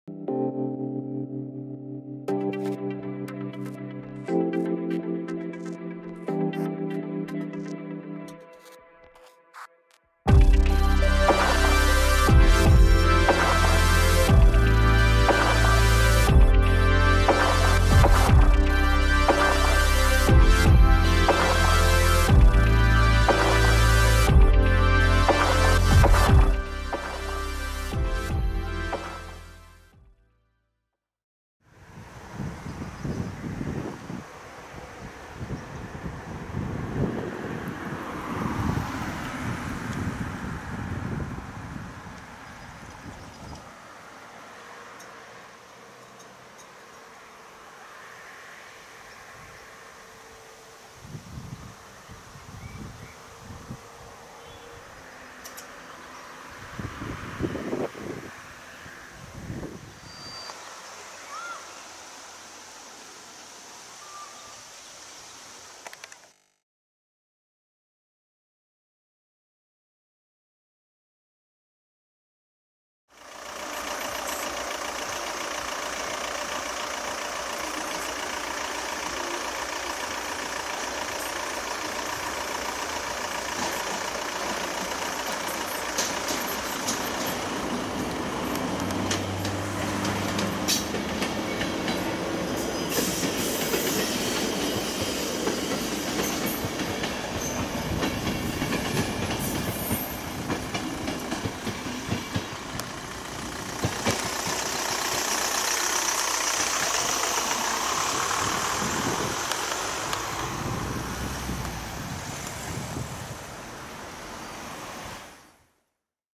EP07-397 z IC Oscypek na przejeździe kolejowym w Bulowicach na ulicy Bielska (S0E5)
Witam,
Dziś nagrałem IC Oscypek na przejeździe kolejowym w Bulowicach na ulicy Bielska. Życzę miłego oglądania!

railcrossing34 2020